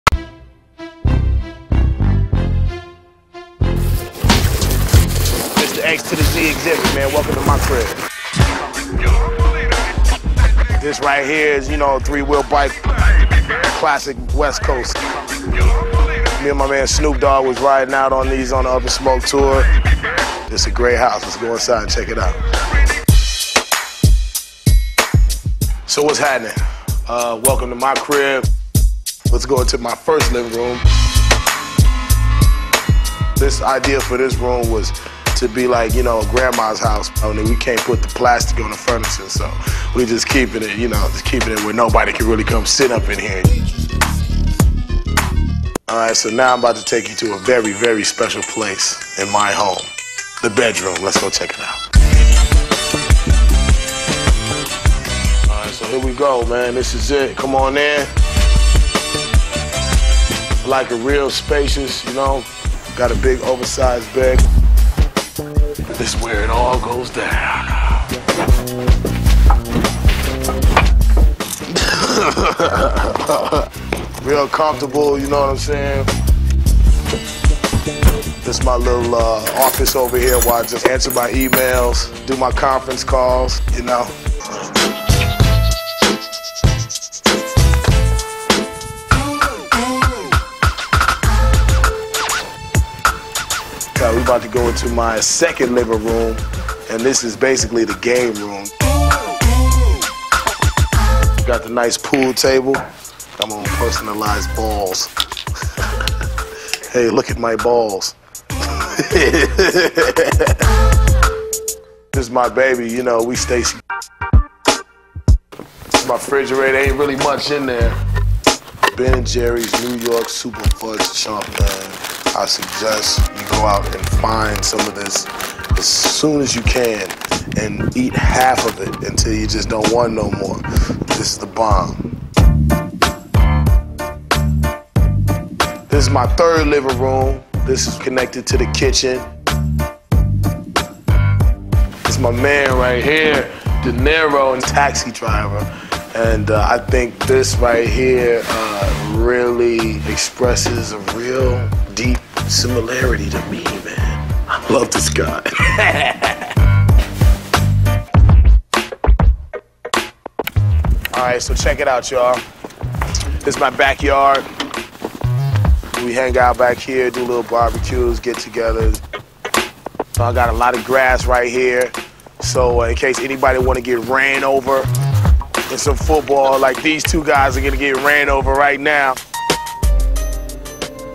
Log House | MTV Cribs Skit
Last year we filmed a video for the log house that we were building and did a skit to mtv cribs. Since the audio of the original video is messed up, I decided to reupload this classical part.